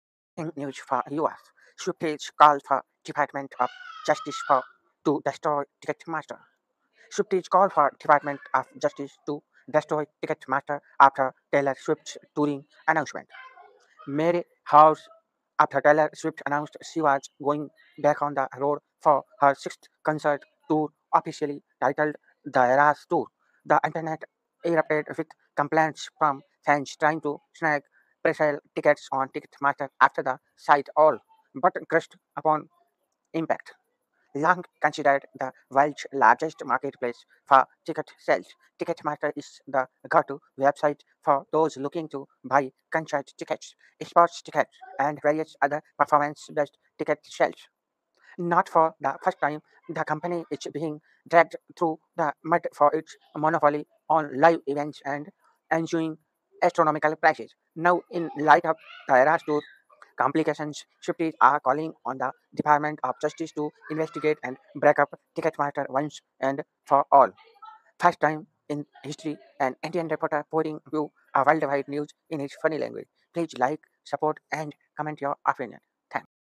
BIG UPDATE| Swifties call for Department of Justice to destroy Ticketmaster| taylor swift
taylor swift lyrics,taylor swift new music,Us news,American news,Taylor swift news,taylor swift album